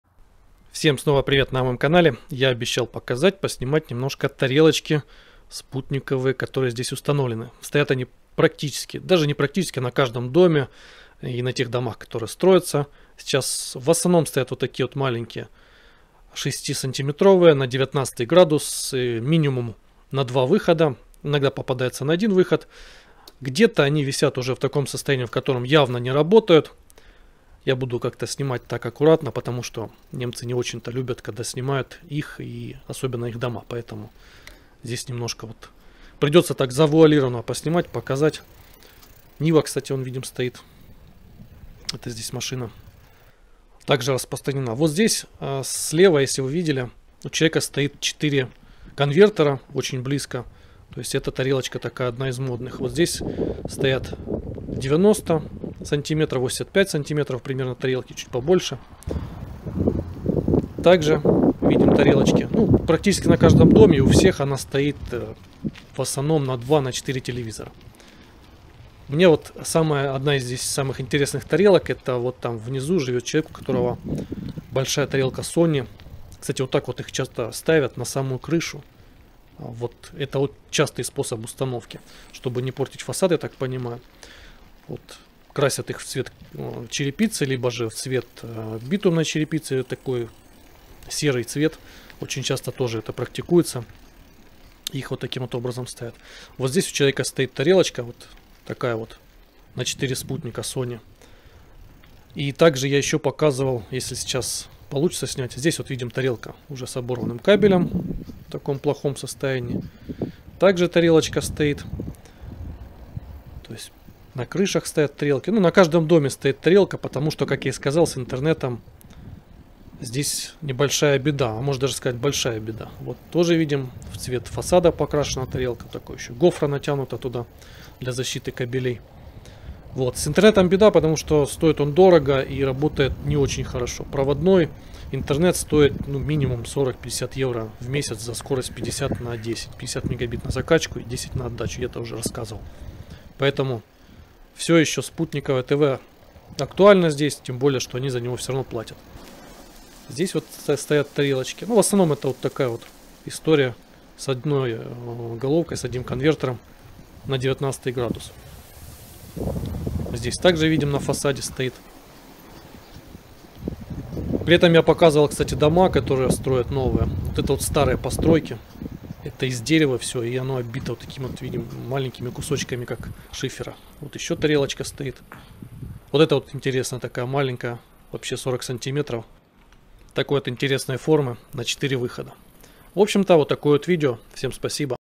Спутниковое тв в Германии 2022. Обзор тарелок немцев и их актуальность в 2022
❤️ КУПИТЬ ТОВАРЫ ИЗ ОБЗОРОВ НА КАНАЛЕ ❤️

▶️ЧТОБЫ ЗАБЫТЬ О ПРОБЛЕМЕ С ИНТЕРНЕТОМ  НУЖНО СМЕНИТЬ РОУТЕР
ХОРОШИЙ РОУТЕР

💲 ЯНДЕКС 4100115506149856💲